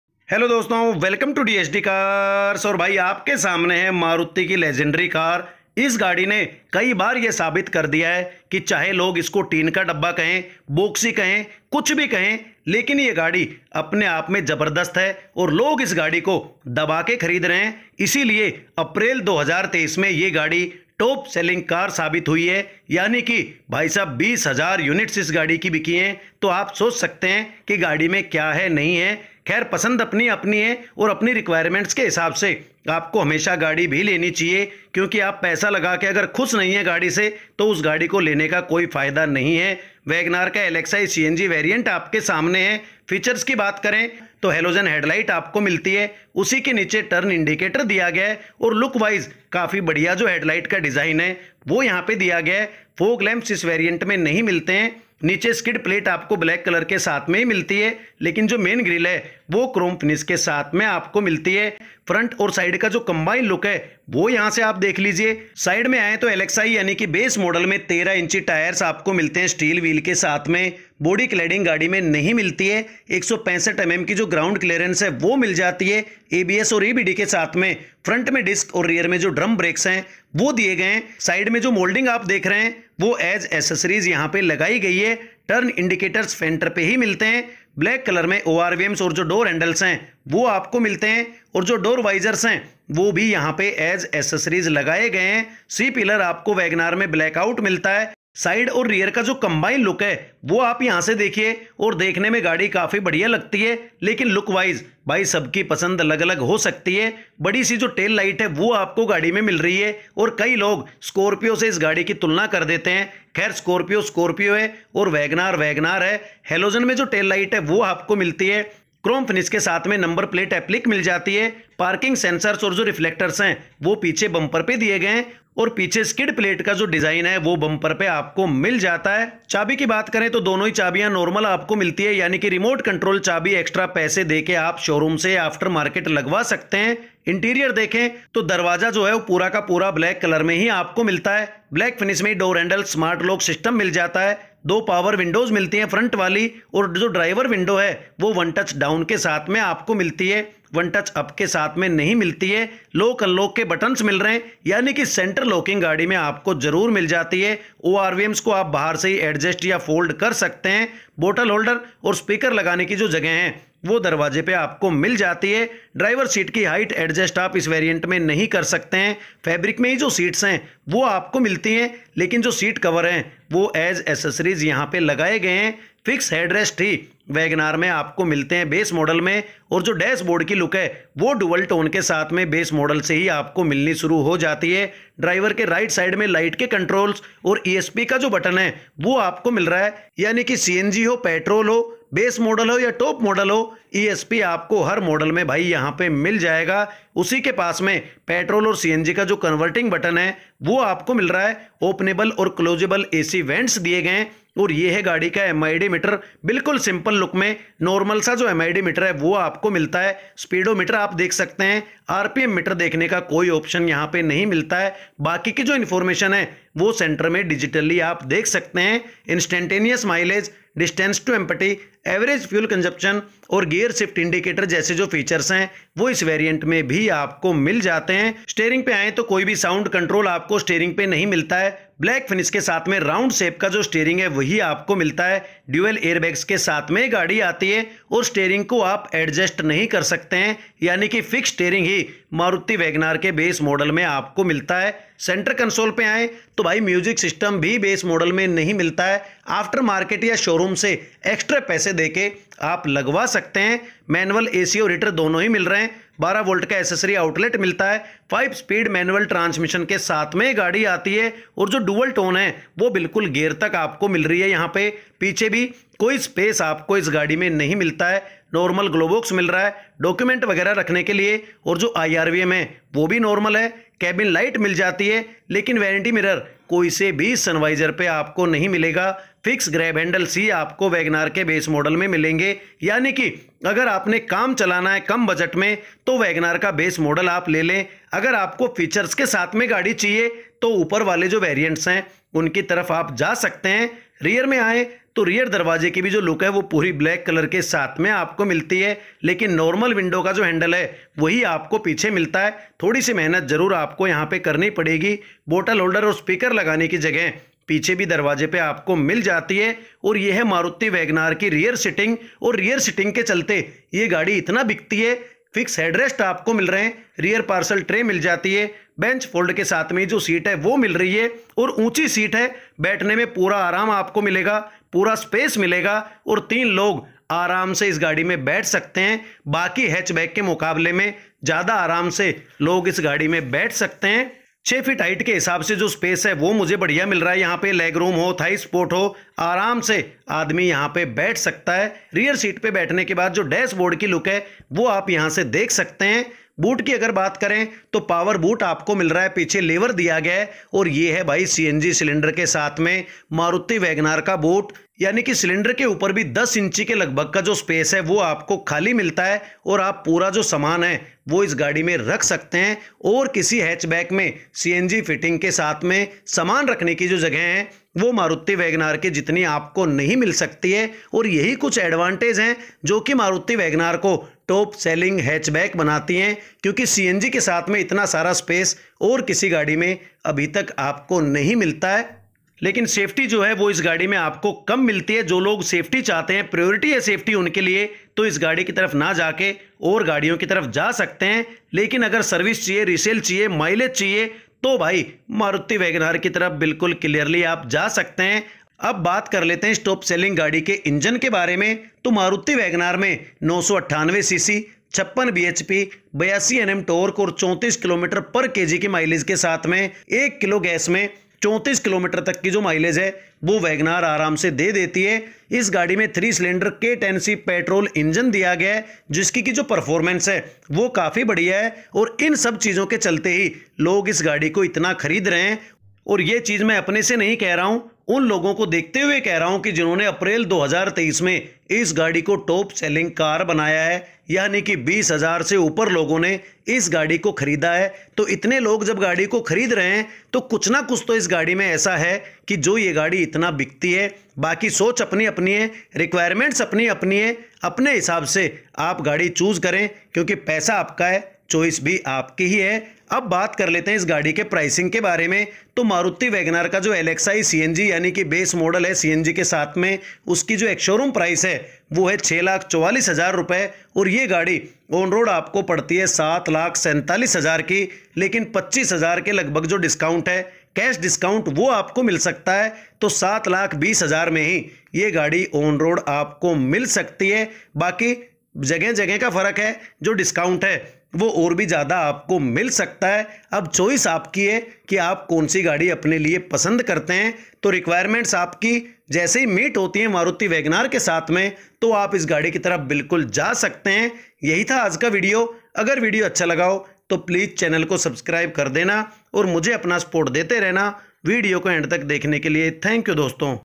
Maruti Wagon r 2023 model डब्बा? साबित कर ही दिया ✅ india's no.1car Wagon r base model lxi cng 2023
Paisa lagane se pahle sab log sochte hain, aur phir gadi lete hain
yani ki 20000 se uper customer jinhone april 2023 me maruti wagon r ko kharida hai
us hisab se is gadi me kuch to baat hai jo ye top selling car in april 2023 bani hai

wagon r ek boxy hatchback car hai jo petrol & cng dono option me aati hai
aaj ke video me wagon r lxi cng 2023 model ka review karenge